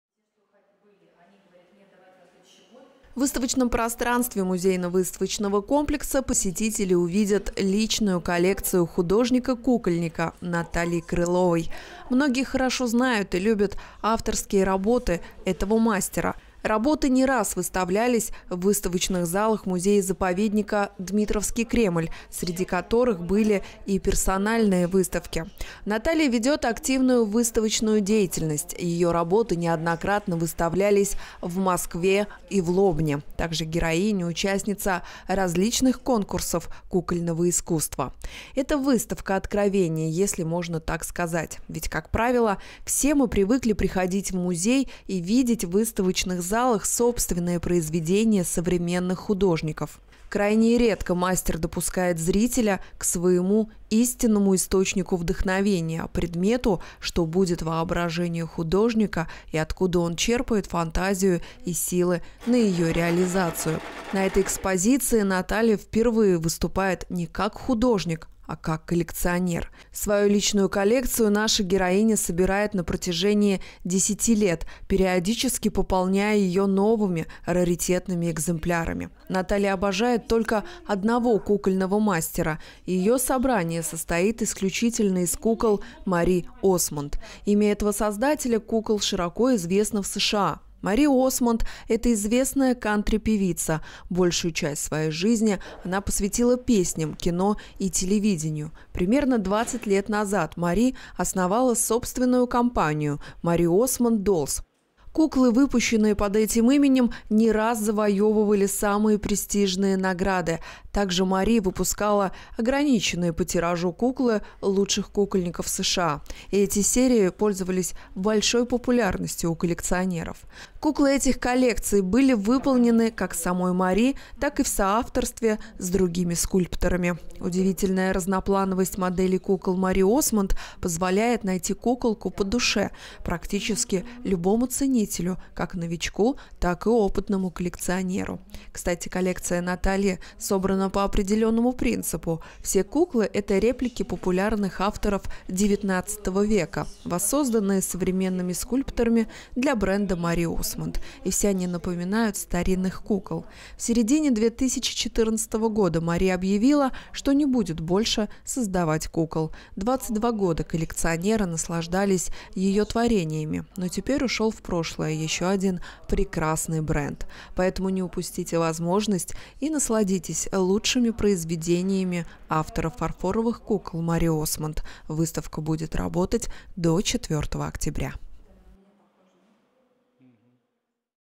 Выставка фарфоровых кукол «Винтажные куклы»
В музейно-выставочном комплексе на улице Загорской открылась выставка фарфоровых кукол Мари Осмонд  «Винтажные куклы». Эту уникальную экспозицию в нашем городе представила художник-кукольник  Наталья Крылова